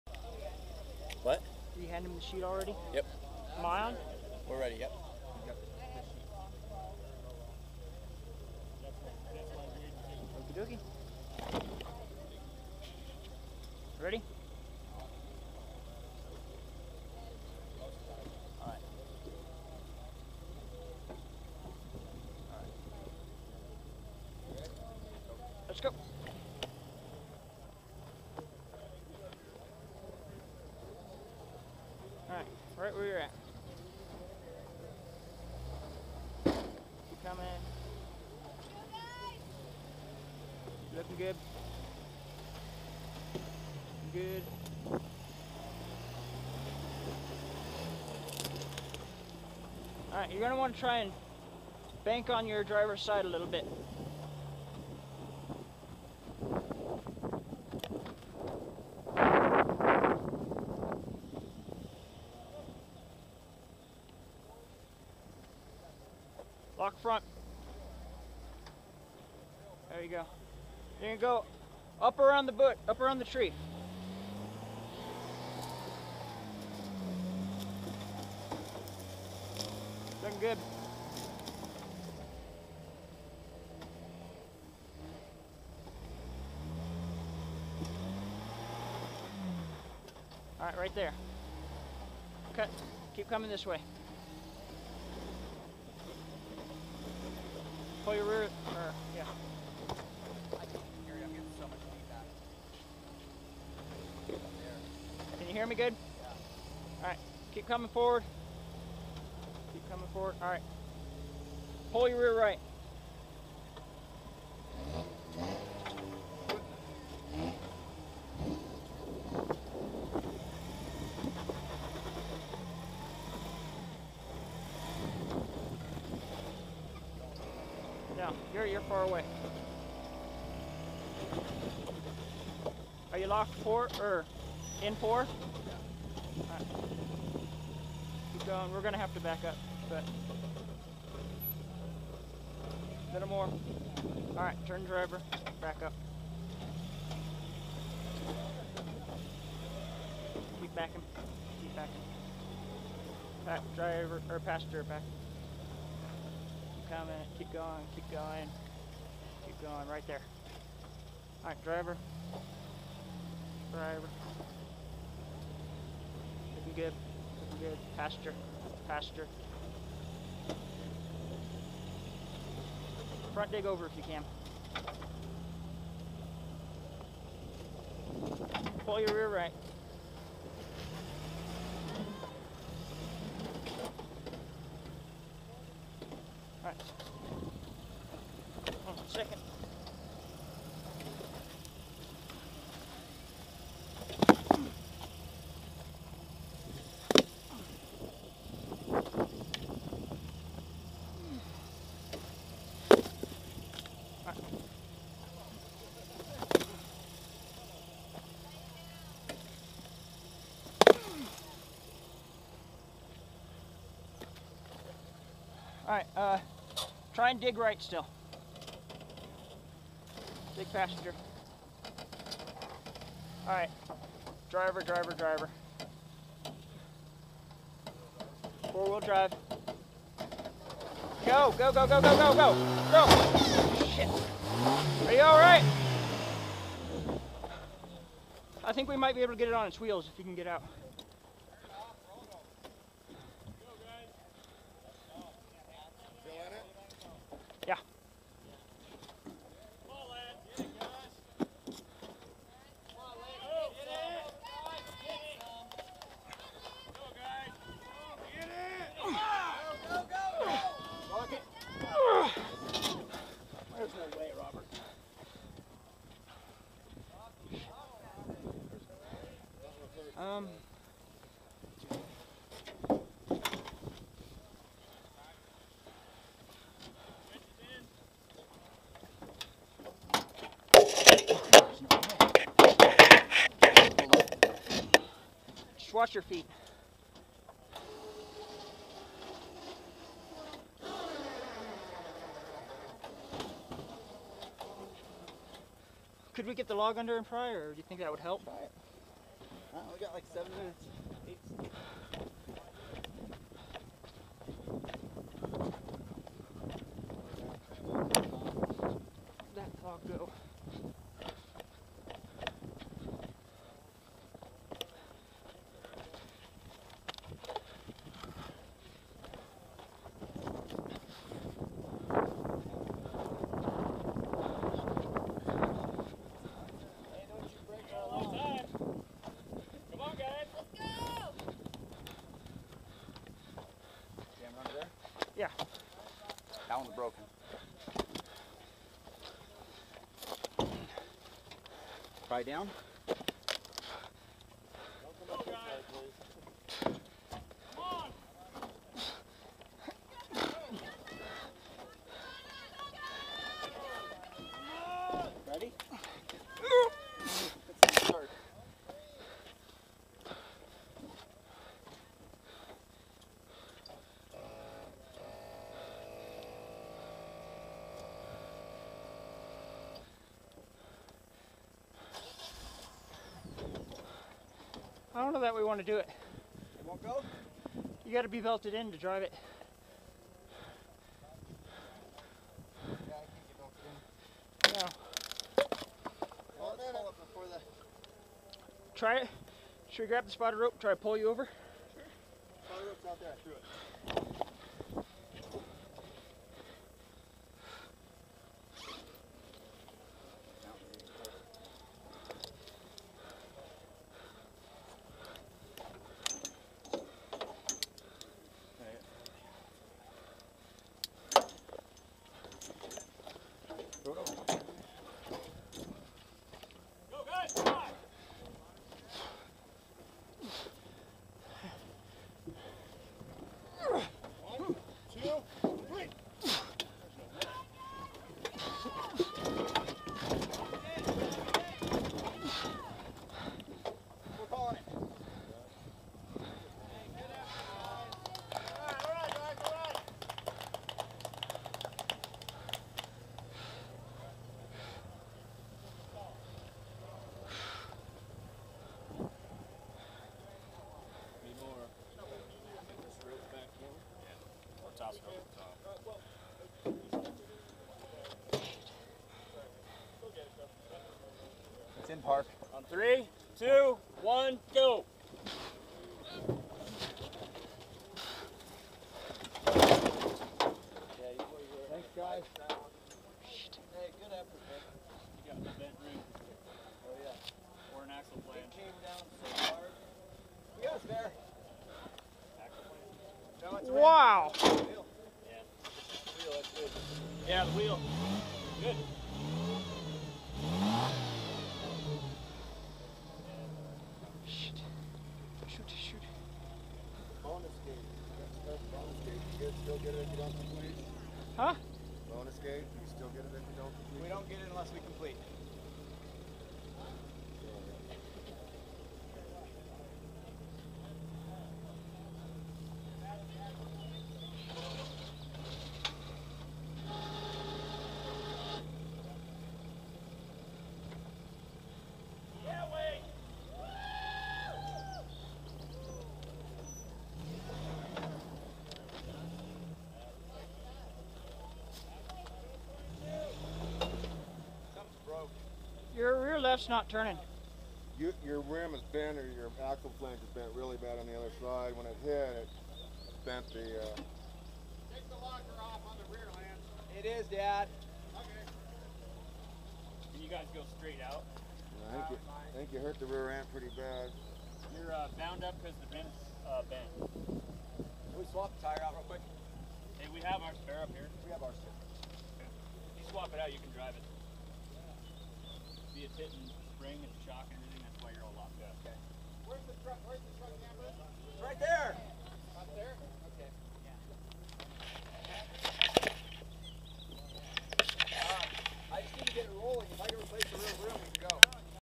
Cal-Neva Extreme July 7th and 8th 2012 Day 2 Course 1 spotter rollover
Longiotti Racing competing at Cal-Neva Extreme Donner Ski Ranch. Round 2 in 2012.